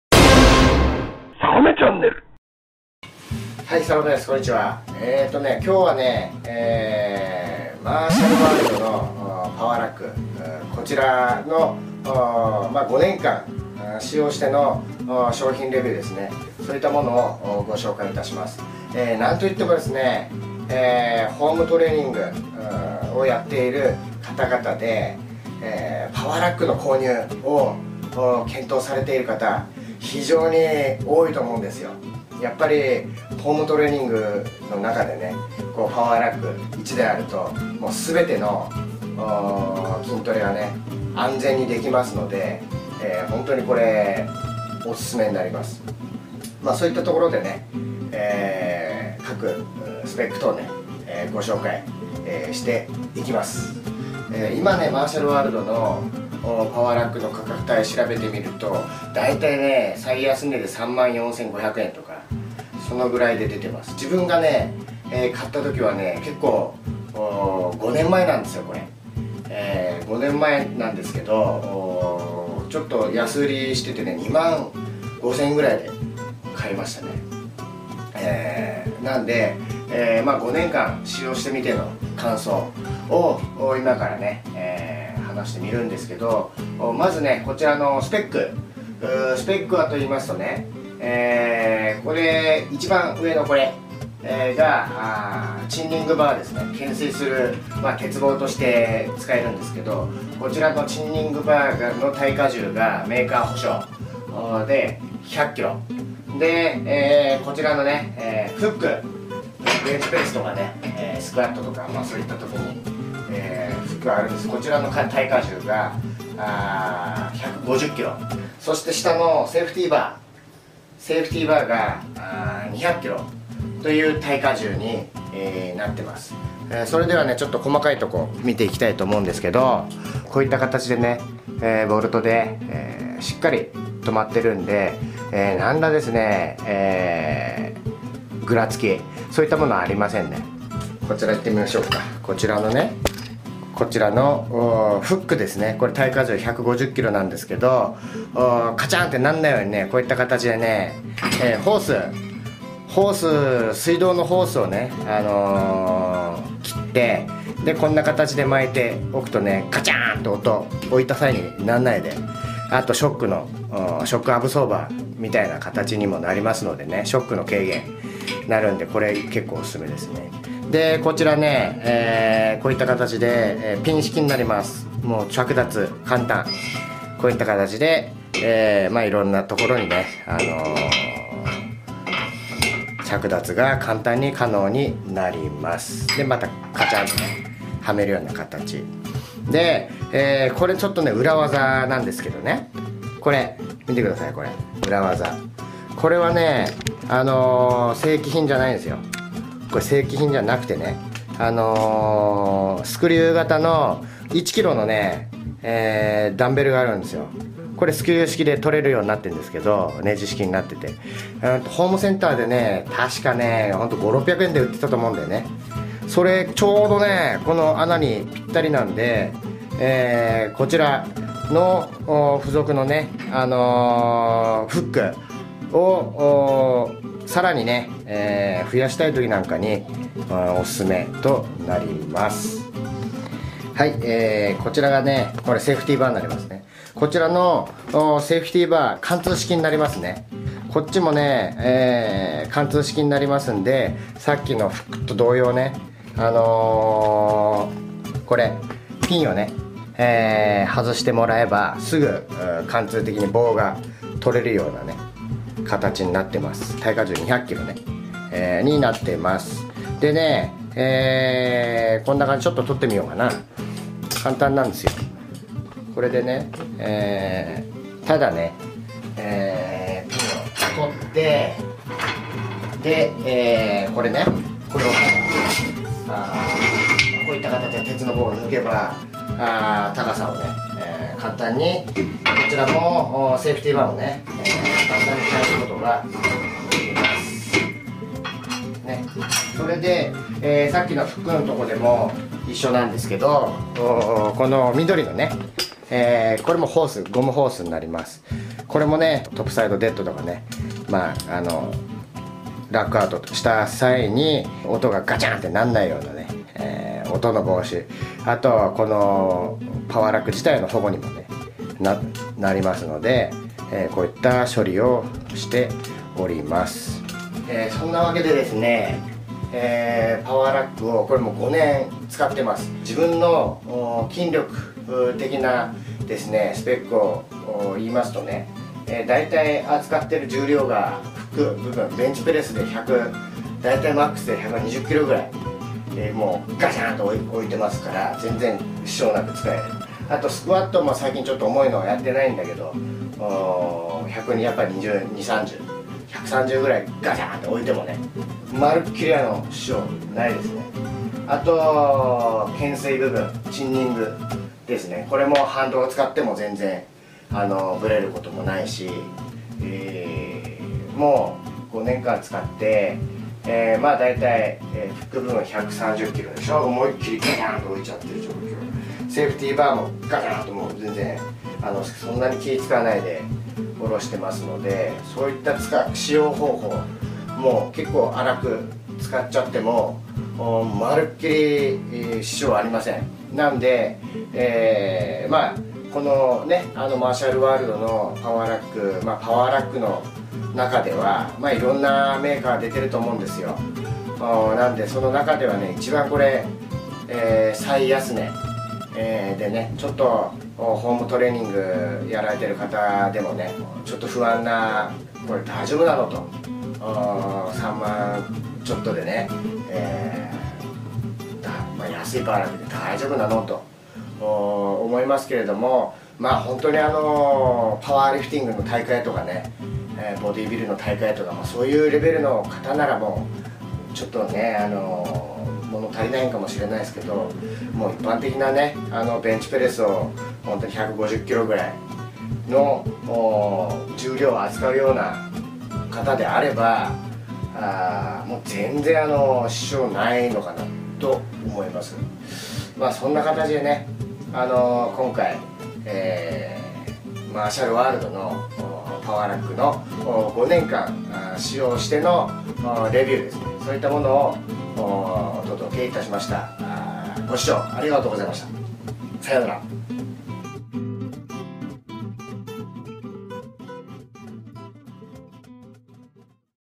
マーシャルワールド スーパーパワーラック５年間使用レビュー
最も安い価格帯のマーシャルワールド社のパワーラックを５年間使用してみての感想です♪
パワーラック購入検討している方は、ご参考までに見てやってください